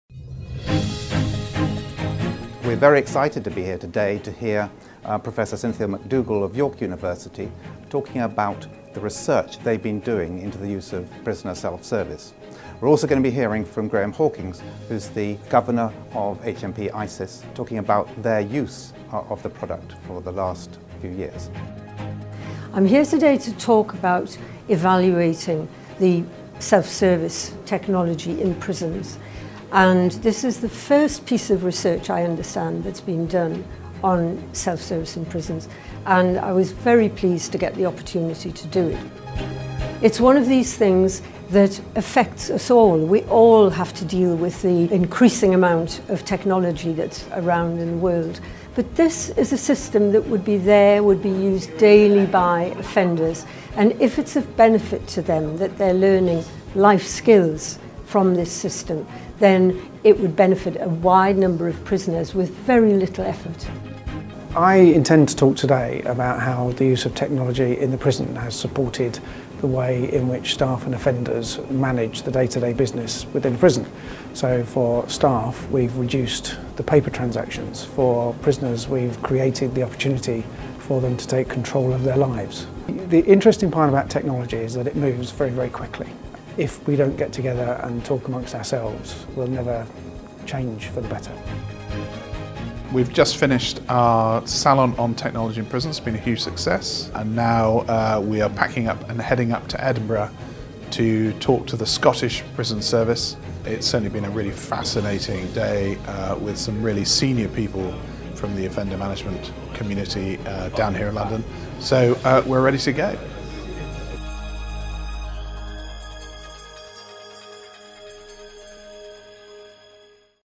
Unilink - Technology In Prisons
From presentation W2-18 - Roger Holding  at ICPA's 17th AGM and Conference, Melbourne, Australia